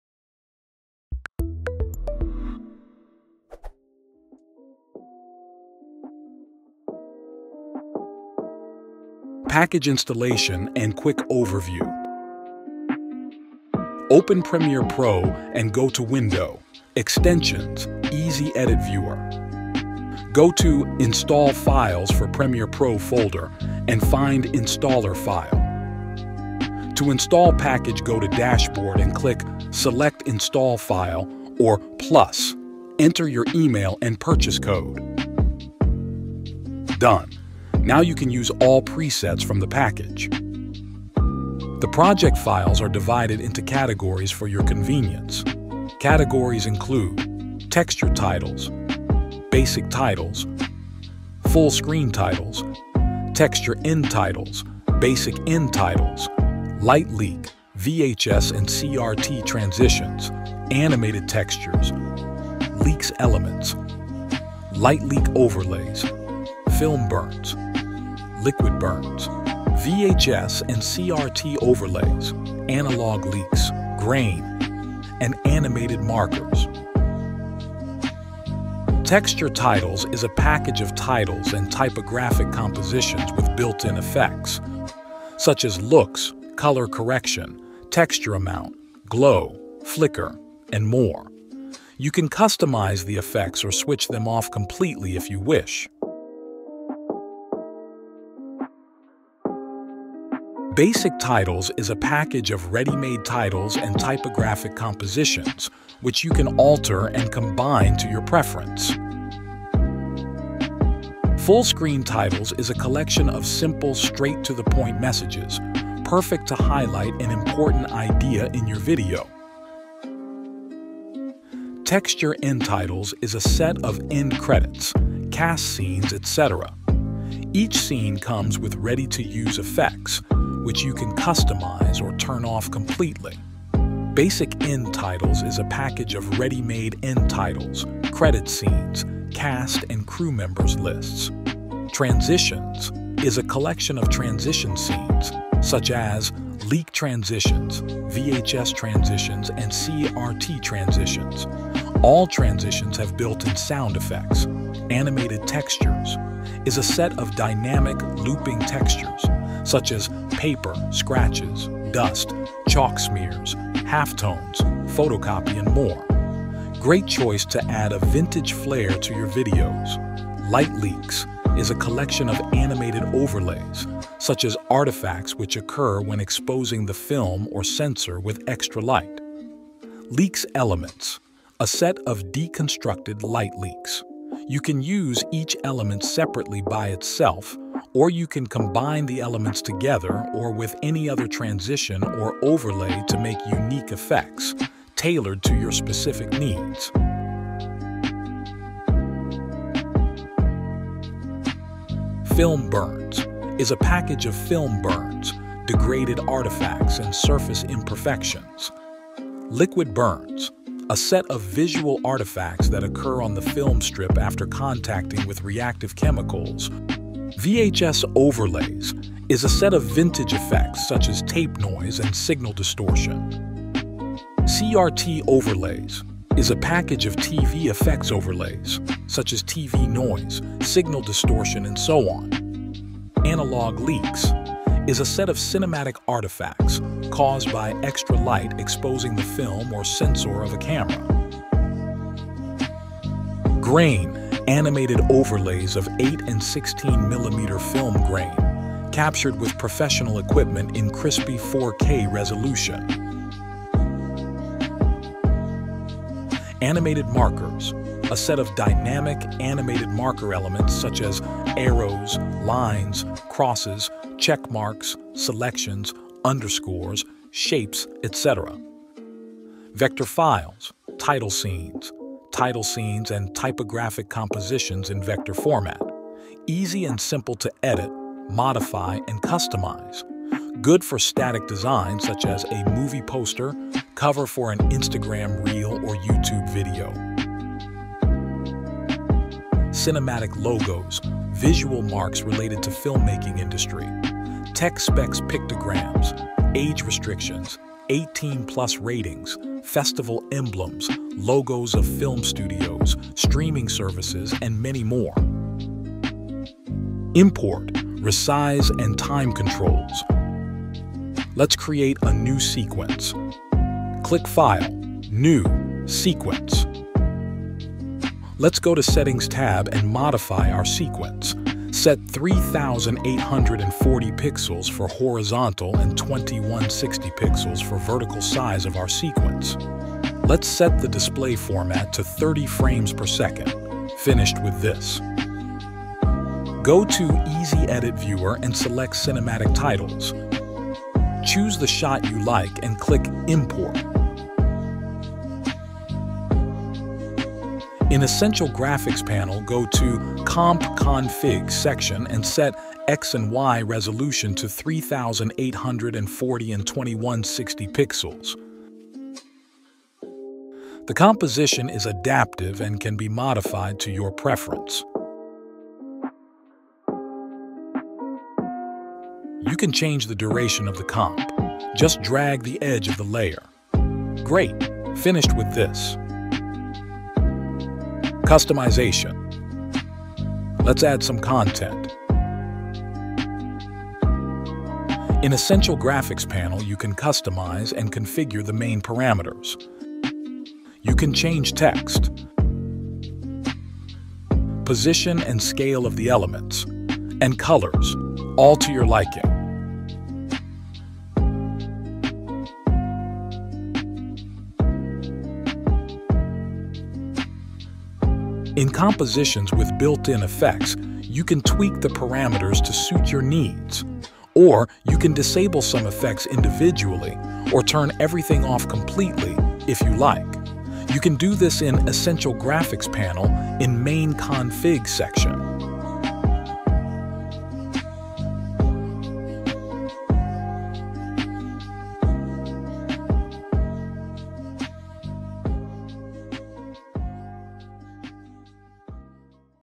How To Use The ESSENTIAL Cinematic Titles in Premiere Pro and Get Your Edits Up!
Turn any video into a movie with Cinematic Titles!

The project includes Basic Titles, Texture End Titles, Light Leaks, Film Burns, Transitions, CRT Overlays, Animated Textures and much more! Choose among 18 categories, including movie credits, cinematic icons, and sound effects to finish the look with details.

Inside you’ll find a wide range of Premiere Pro templates, prepared to become a part of your movie trailer, film intro, indie music clip, or digital poster! Each template supports 4K to make your title sharp on a big screen.

Spend your time on creativity - the customization is easy for beginners. Mix&Match titles with overlays and backgrounds to achieve a unique design. Any combination will match!

Customize any color, easily change text, and resize the composition to fill your needs. You can auto-resize the templates to vertical resolution and announce your films on social media.

Add some crisp to your clip with 8mm & 16mm film grain, TV noise, and signal distortion.
Finally, a cherry on the pie - use vector files to create your own cinematic title and logo!

 Cinematic Titles will make any video look like a high-quality cinematic production!

TIMING:
00:00 - Intro
00:13 - Installation
00:41 - Overview
04:28 - Vector Files: Overview
05:35 - Import
05:47 - Resize
05:59 - Time Controls
06:17 - Customization
06:19 - Adding Content
06:47 - Built-in Effects